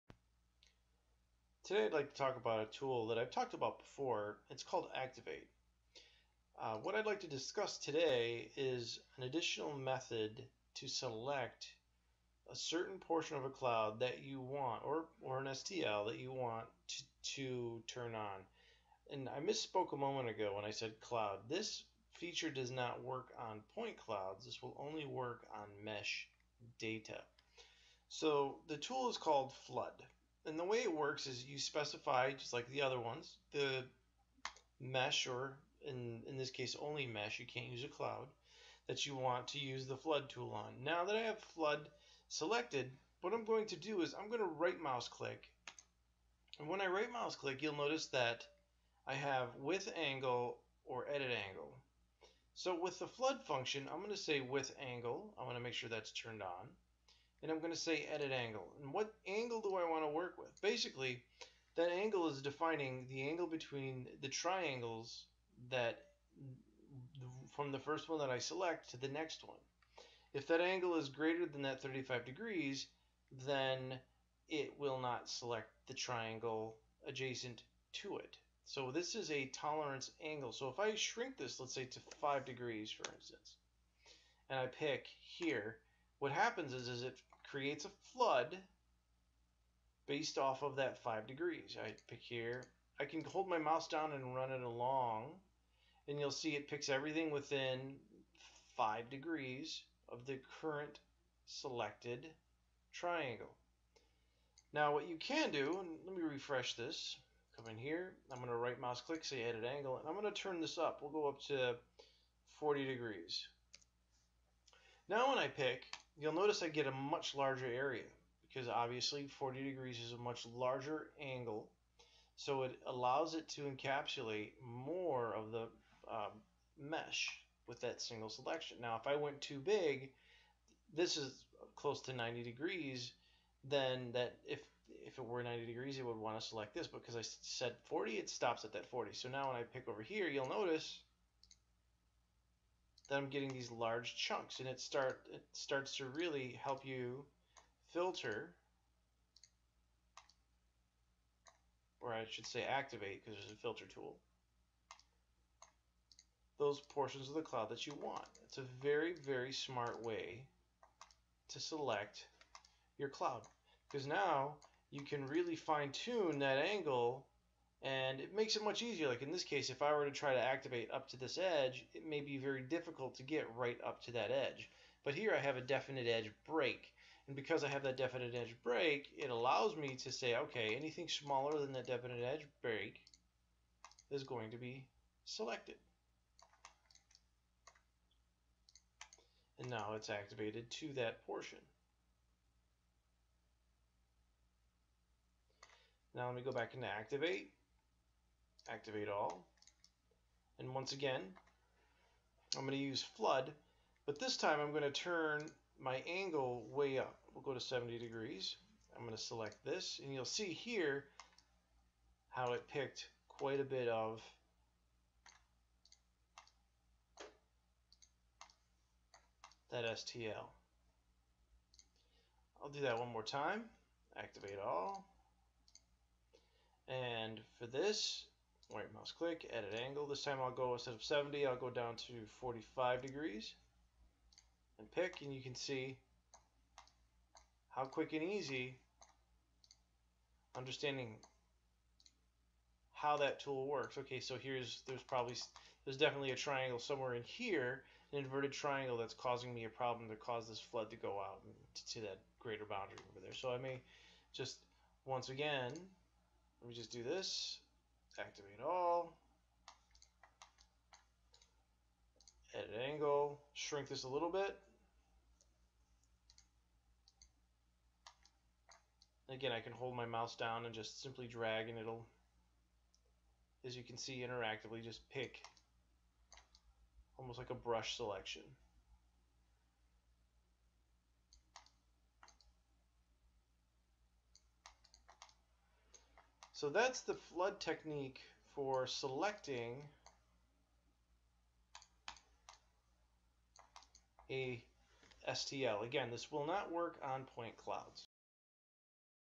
Flood Selection | Digitized Shape Editor | CATIA
An easy method to select portions of an STL mesh that you want to work with.